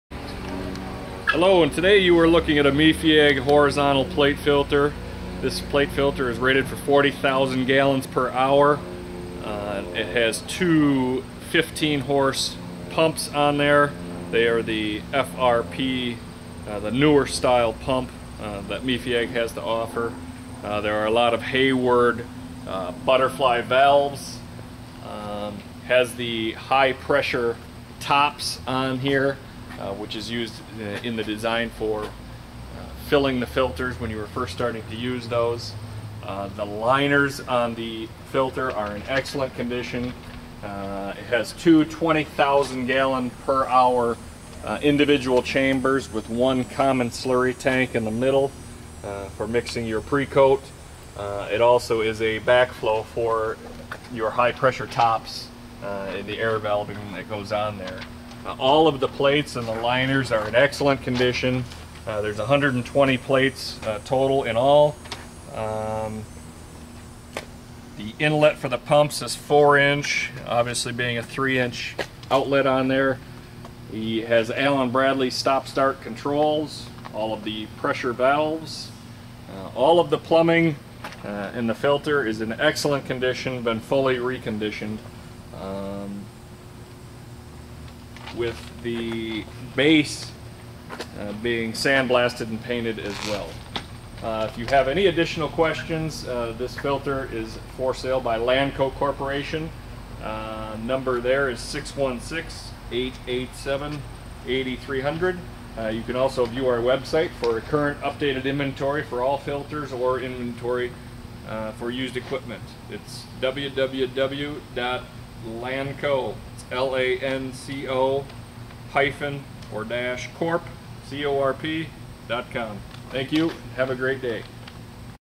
Mefiag 40,000 GPH Horizontal Plate Filter F2025C
F2025C  Mefiag 40,000 GPH Filter
Manufacturer       Mefiag
Model #       40000-HFD-SY
Style       Horizontal Plate
GPH       40000
Pump HP       (2) 15
Number Plates       120
Dual Allen Bradley Start/Stop * Filtration Surface Area: 285 Sq. Ft * Max Operating Temp: 190
 Degrees * Filter Aid Capacity: 35-50 lb * Powdered Carbon Capacity: 9 lb * Sludge Holding Capacity:
 8 Cubic Ft. * Inlet/Outlet Connections: 4"/3" * Two 15HP Motors